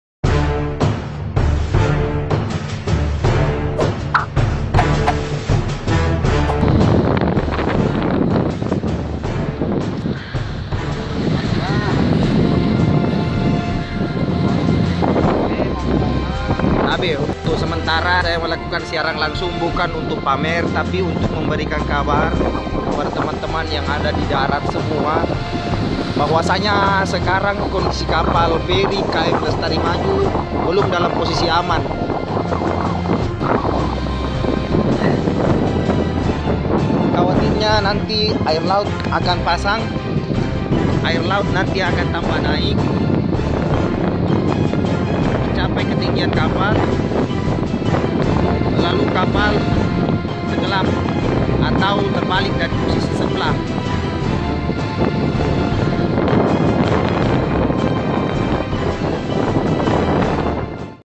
Penumpang KM Lestari Maju Sempat Live di Medsos Sebelum Karam
Salah seorang penumpang KM Lestari Maju ini begitu nekat melakukan siaran langsung di akun facebook (FB) miliknya
saat detik-detik kapal KM Lestari Maju karam diperairan Kepulauan Selayar.